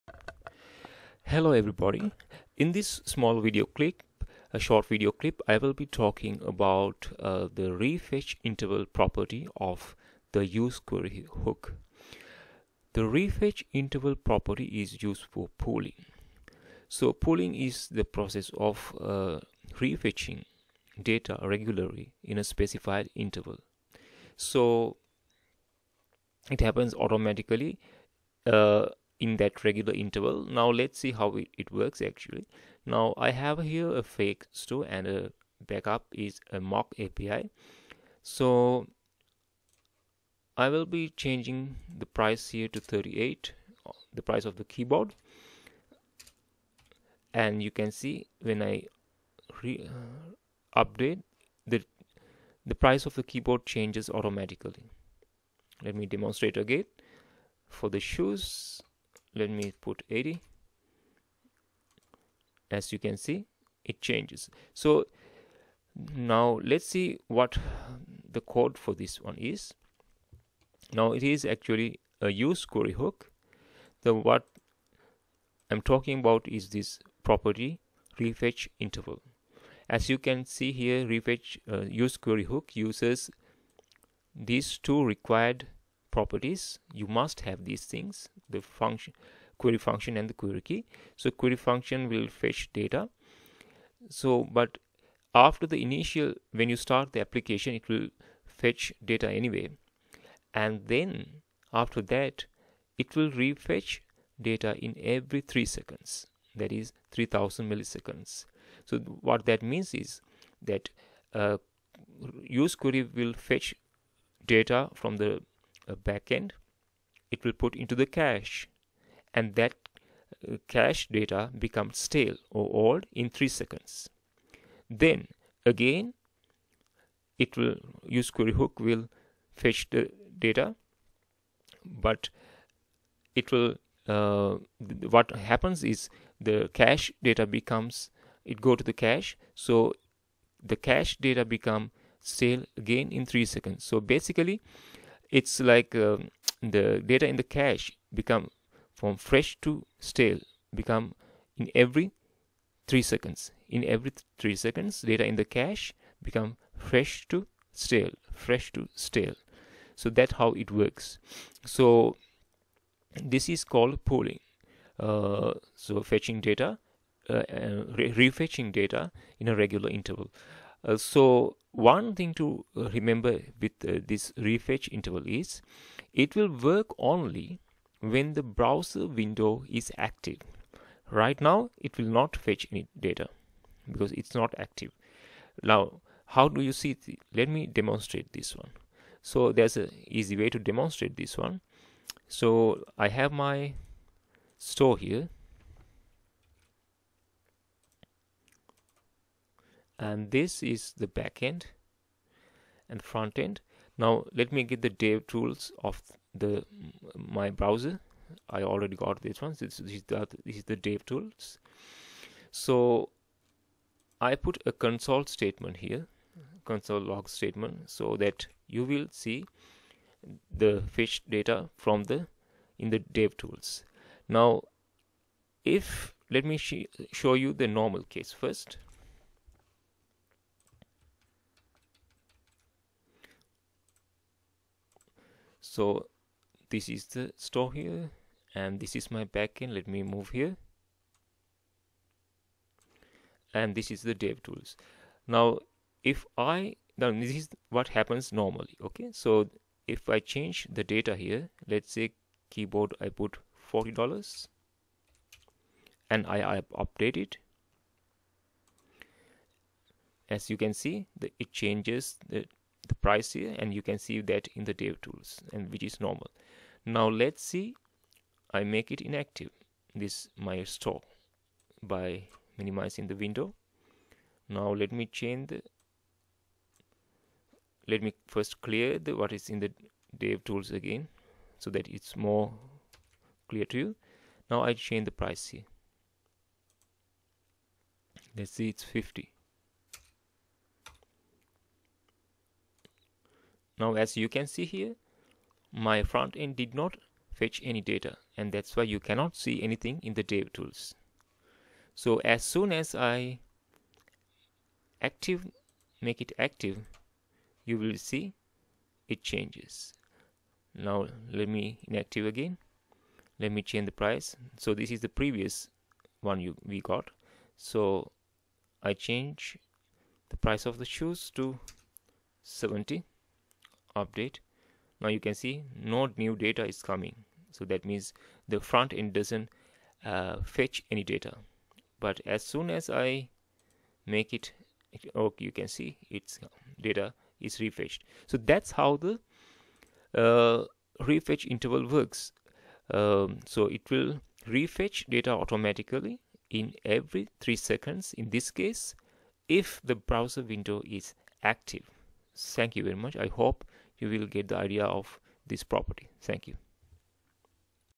How to use refetchInterval option of UseQuery hook in React Query
This video demonstrates the use of the refetchInterval property of the useQuery hook in React Query. React Query is a library used for server state management in React apps